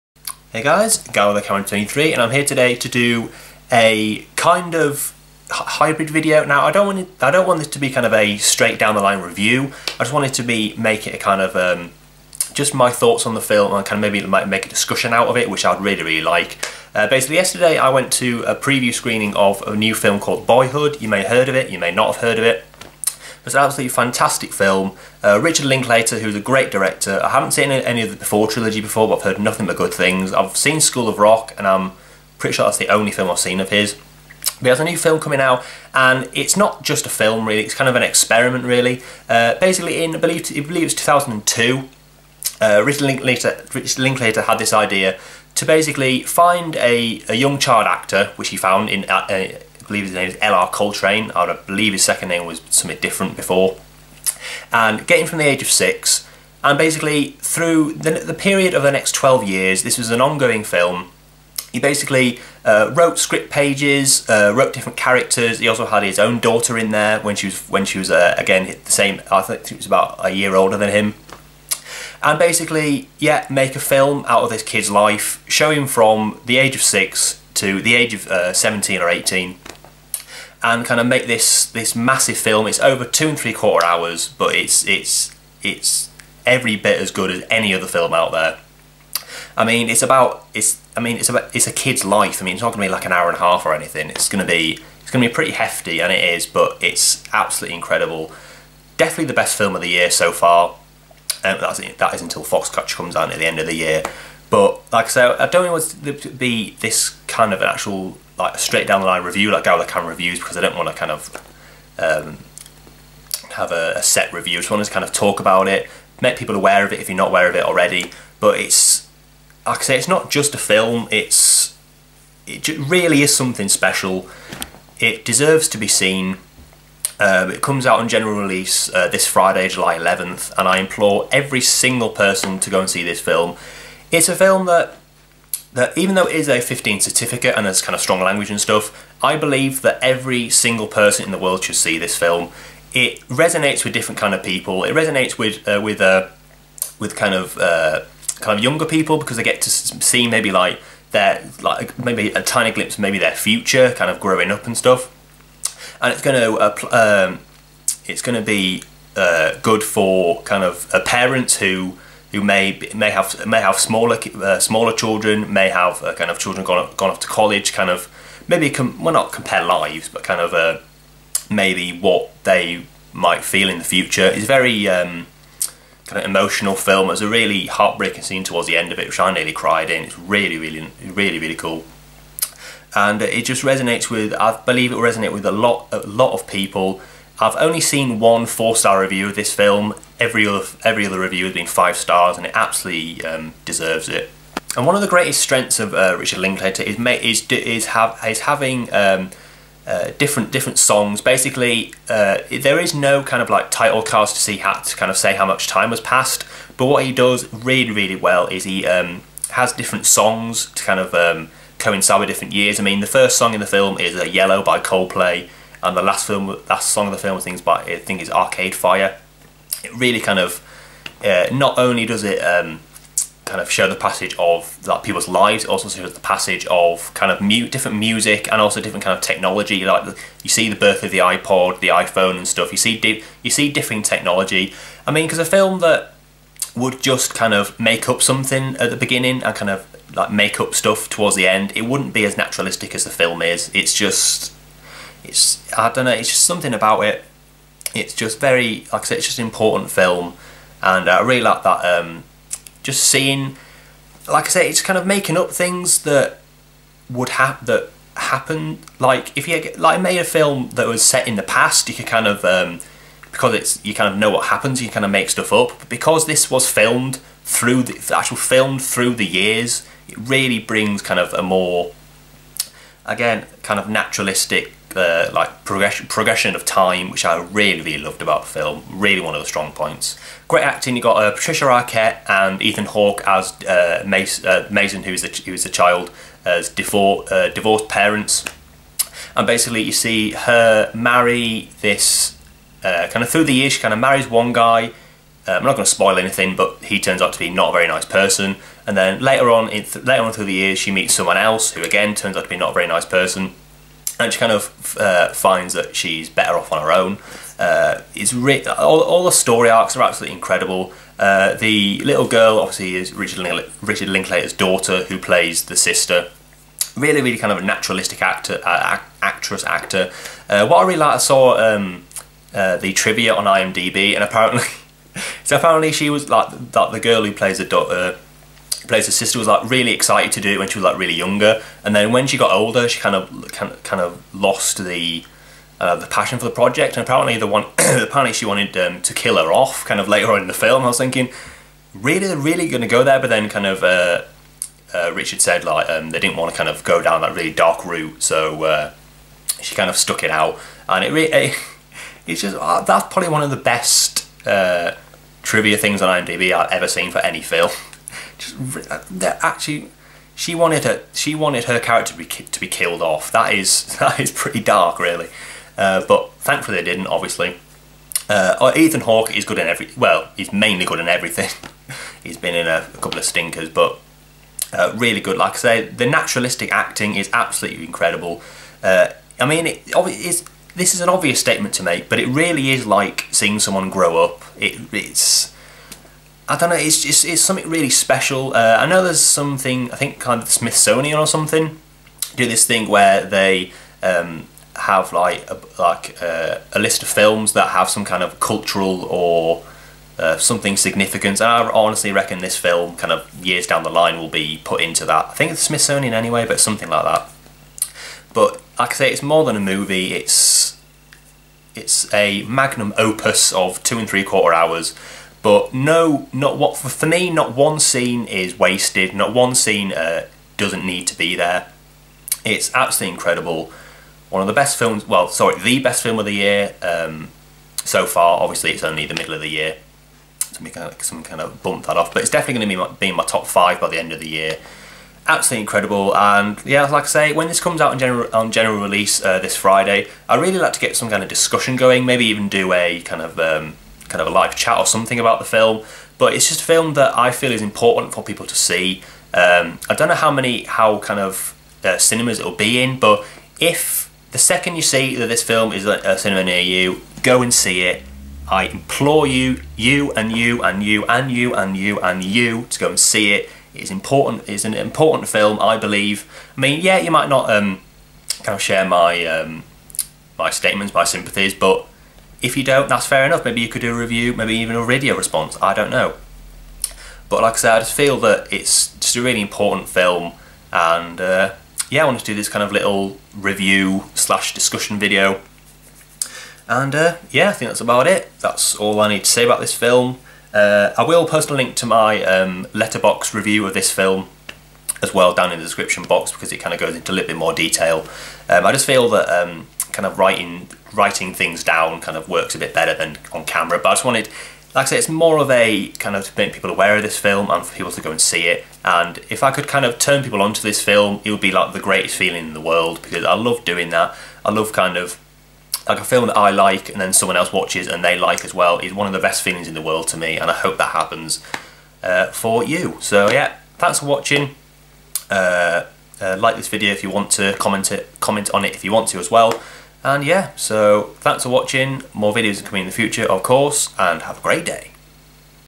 Boyhood (2014) Review/Discussion
In this video I review Richard Linklater's latest masterpiece Boyhood, a film 12 years in the making.
I would love to maybe get a discussion going as I believe it is a very important film.
letterboxd Review-letterboxd.com/melonhed/film/boyhood

Add me on Facebook-Peter Fletcher